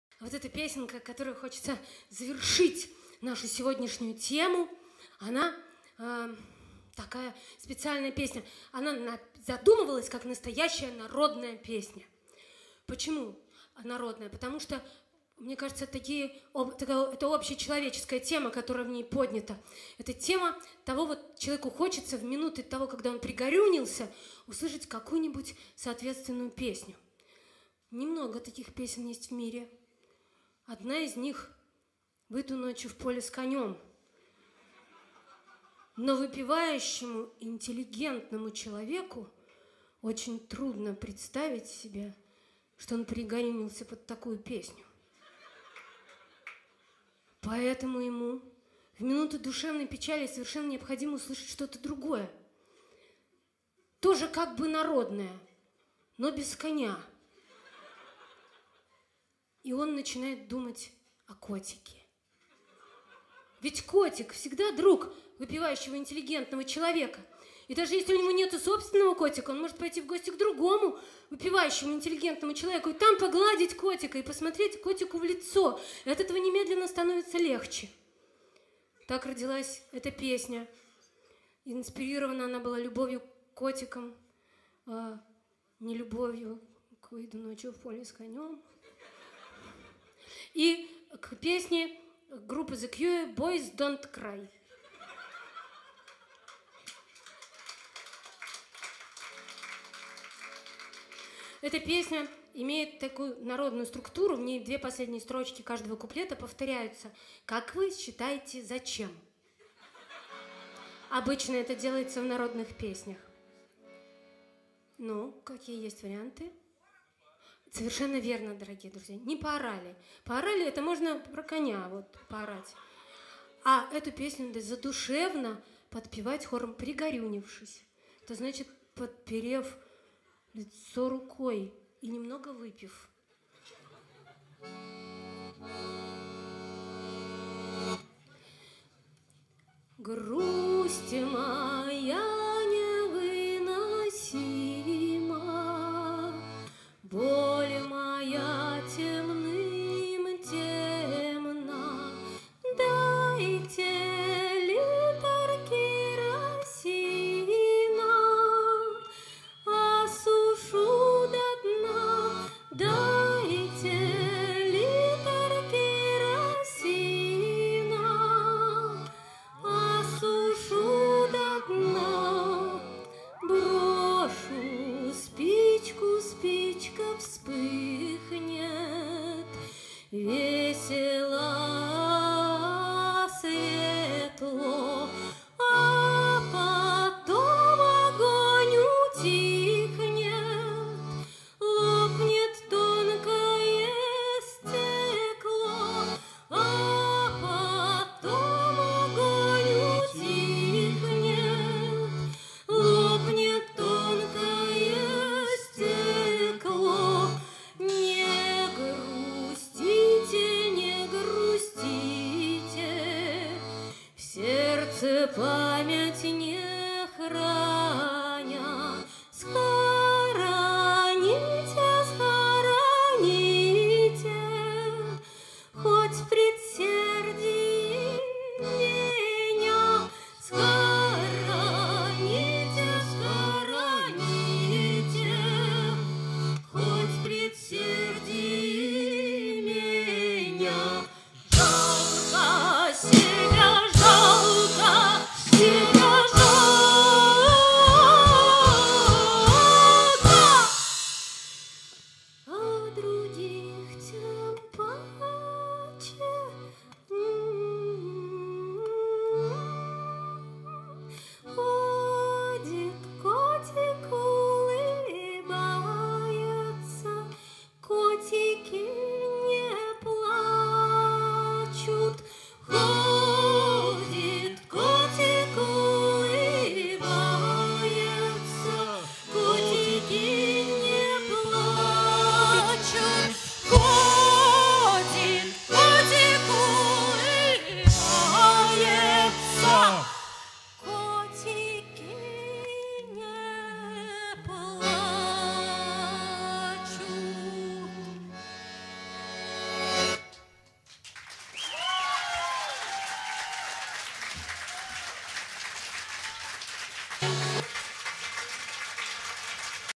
МИКРОКАБАРЕ. Котики не плачут.
"МИКРОКАБАРЕ" с программой "Котики не плачут". Это первое клубное выступление коллектива в Новосибирске. Состав коллектива: Света Бень — вокал, концертина, укулеле и Артем Залесский -барабаны ,электроника.
22 августа 2019. Кабаре Кафе "Бродячая Собака" Новосибирск. Собаке 13 лет